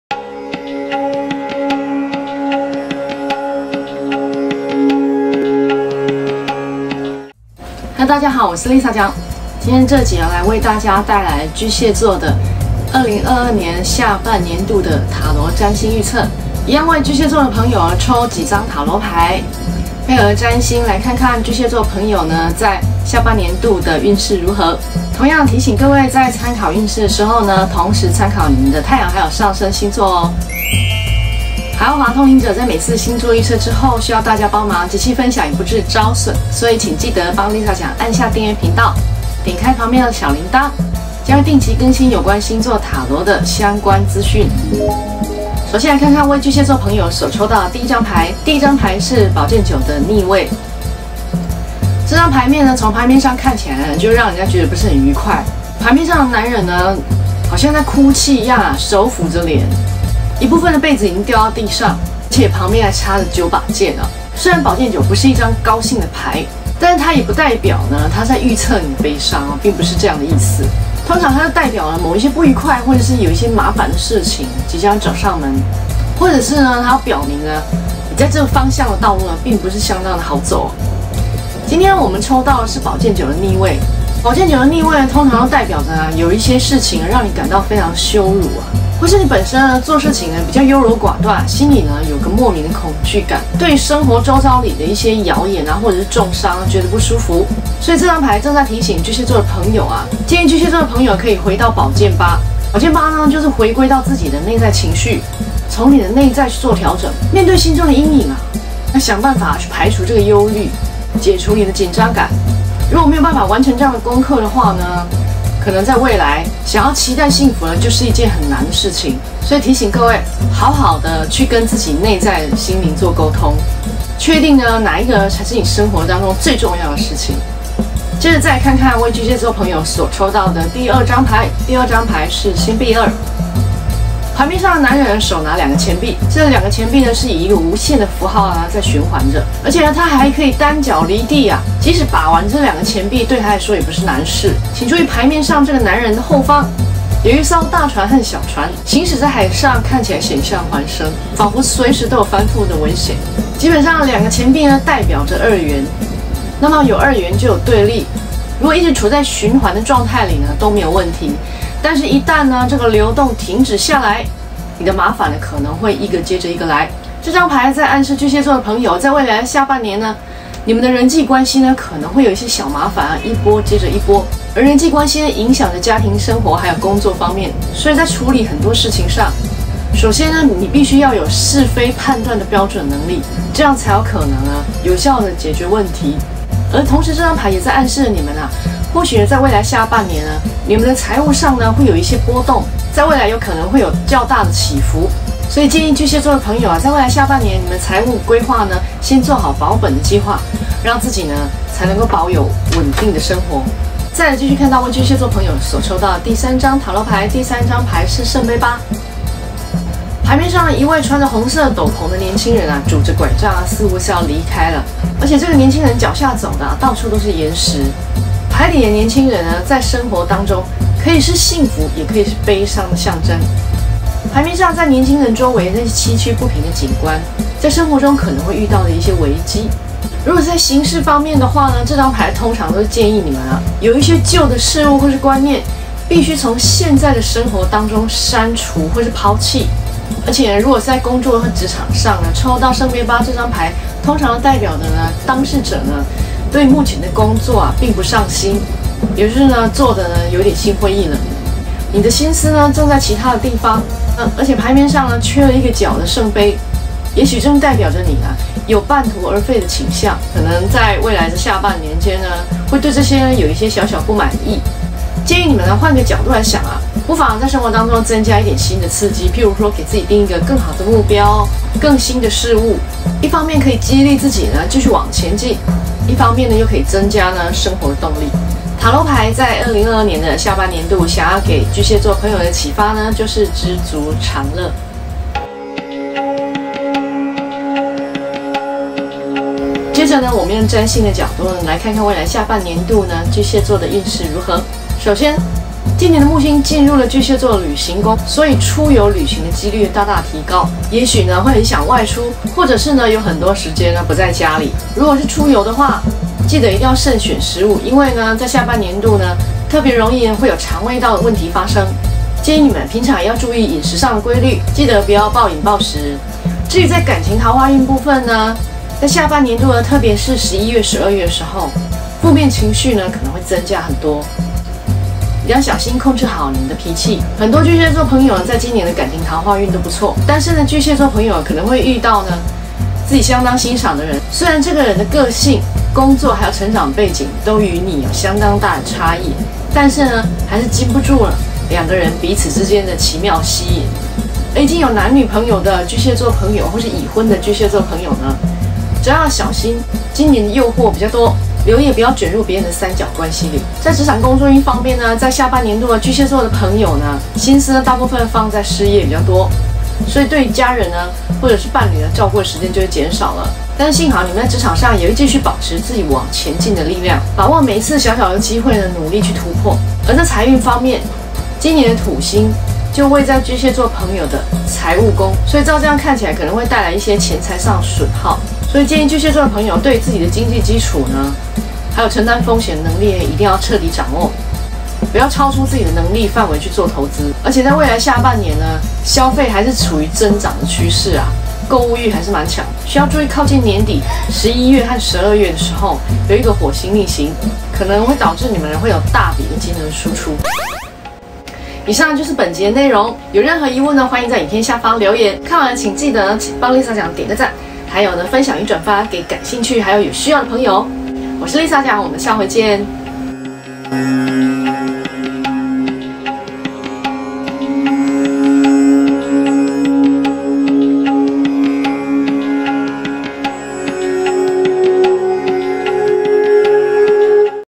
2022下半年巨蟹座塔羅占星運勢預測 :桃花運逆轉 事業人際關係貓膩多...【海奧華通靈者】
2022下半年的巨蟹座財運&桃花運波折較多，考驗著個人內在心靈 與各方面的修養#巨蟹座#塔羅牌#占星
✡✡✡✡✡✡  訂閱理沙醬 點開鈴鐺 分享&轉發 ✡✡✡✡✡✡
     看完影片點個讚是做功德
分享影片本宮也會很感謝DER~